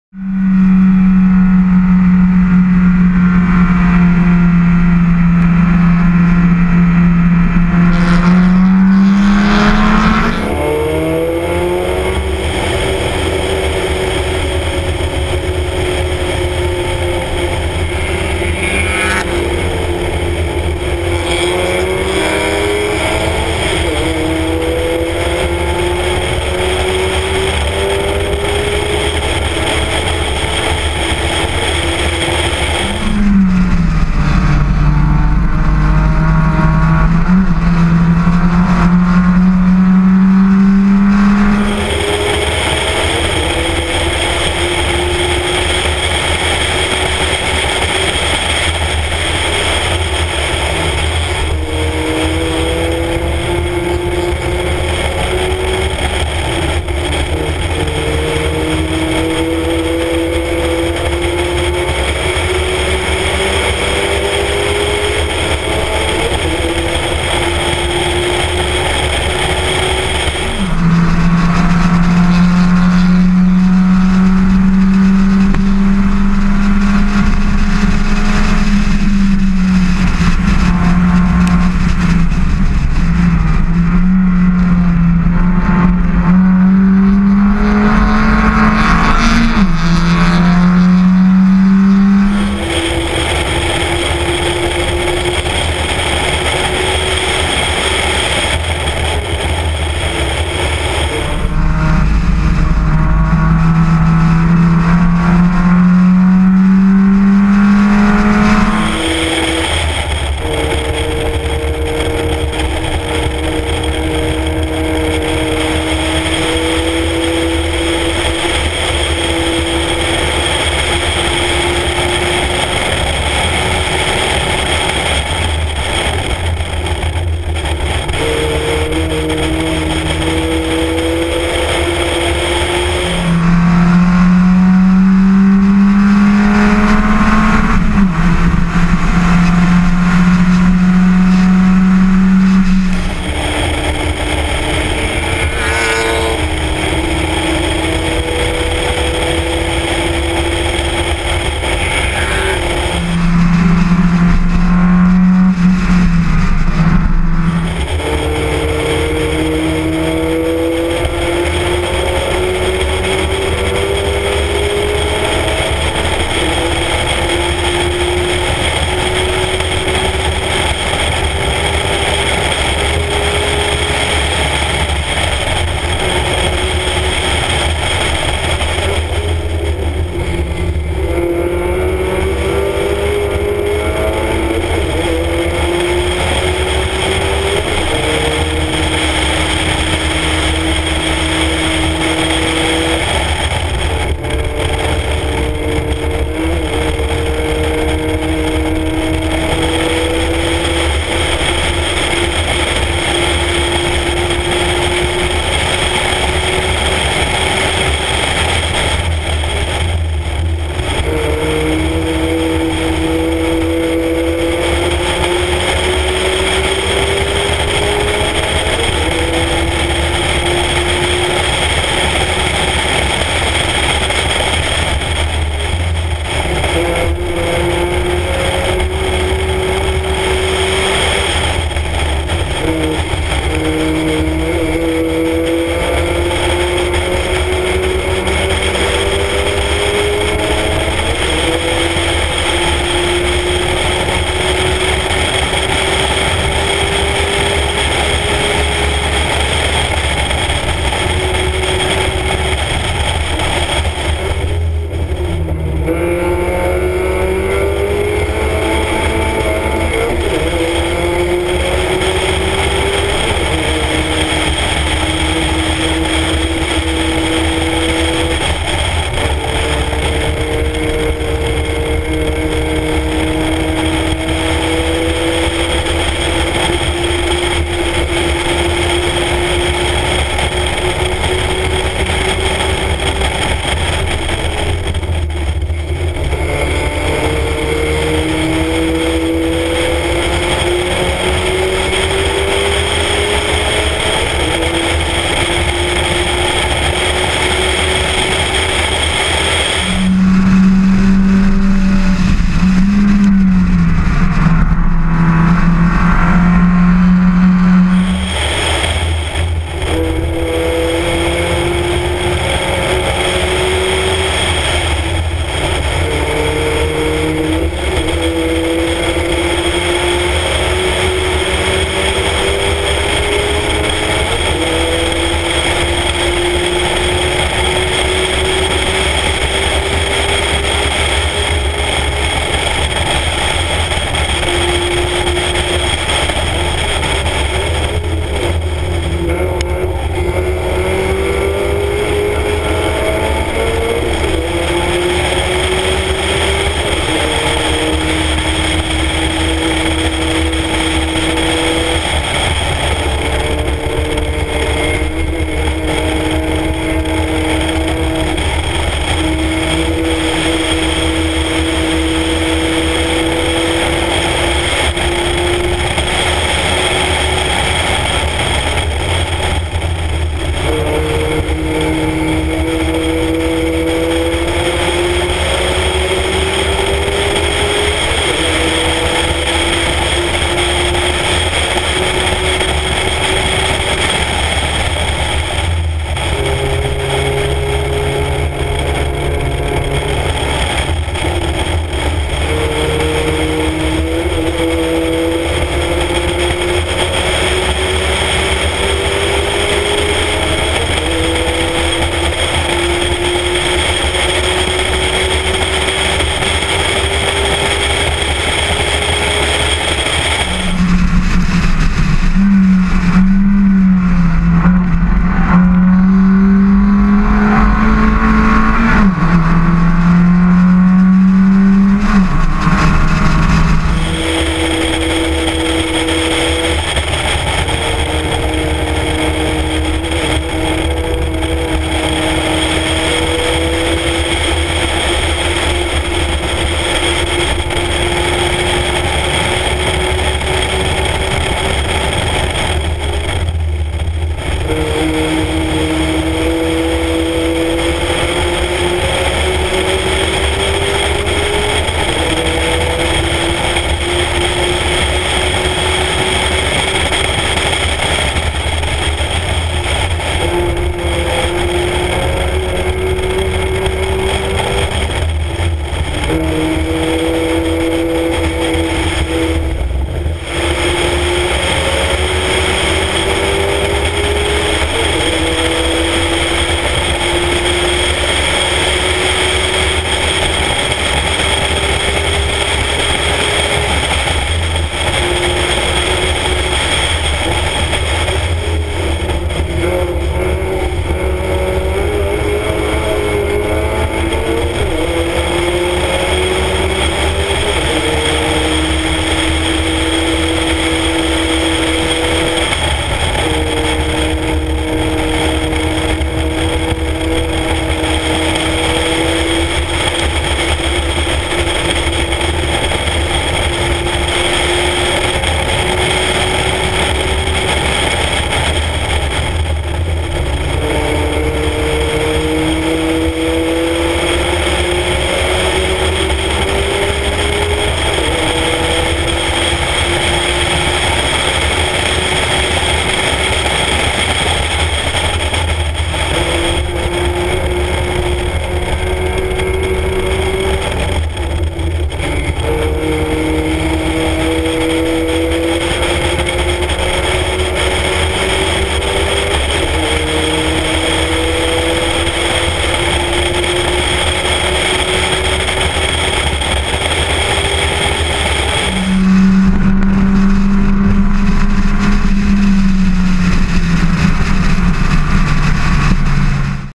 Miata Tuning Autobahn
Spec Miata Test 'n tune at Autobahn Country Club North on 7/13/13.